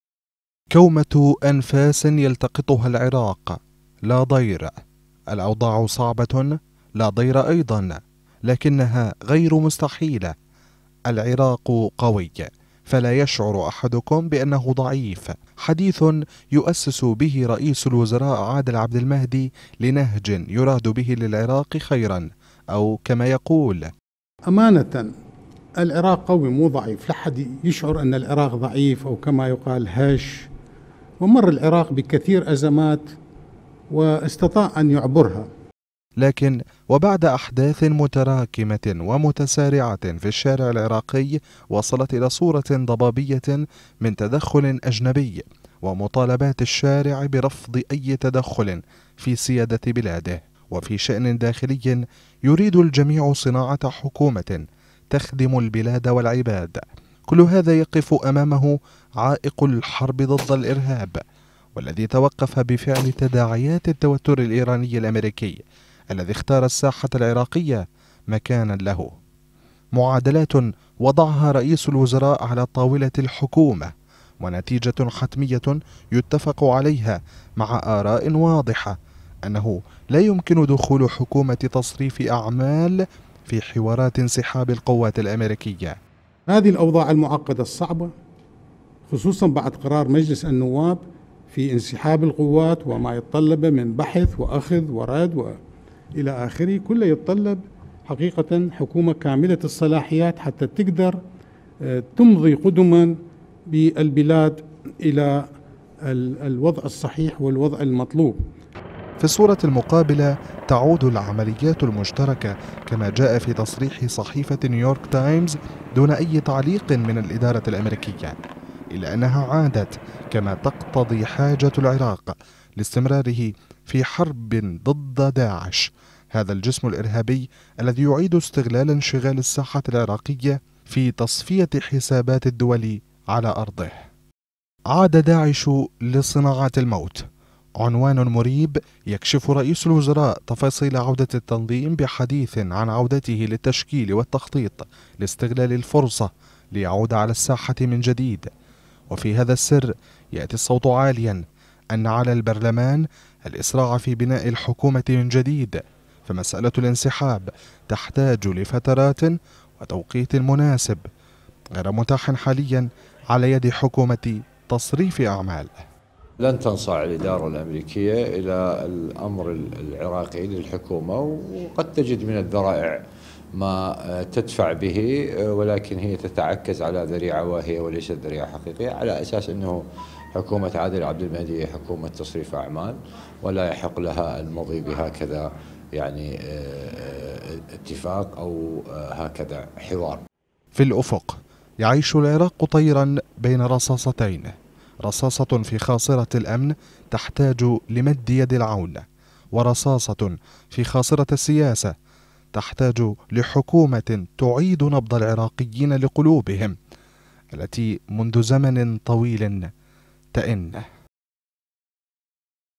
العراق بين انسحاب القوات الاميركية وعودة داعش | تقرير
بعد أن طالب مجلس البرلمان، بانسحاب القوات الأميركية، وجدت حكومة تصريف الأعمال نفسها، أمام معضلة عدم مقدرتها على إقرار مطلب يقضي بوجود حكومة فاعلة على الأرض... إلى جانب حديث صحف عالمية عن عودة العمليات المشتركة في ظل مخاوف عودة تنظيم داعش على الأرض، قراءة في حديث رئيس الوزراء عادل عبد المهدي حول مسألة انسحاب القوات الأميركية وما يحتاجه العراق في الوضع الراهن .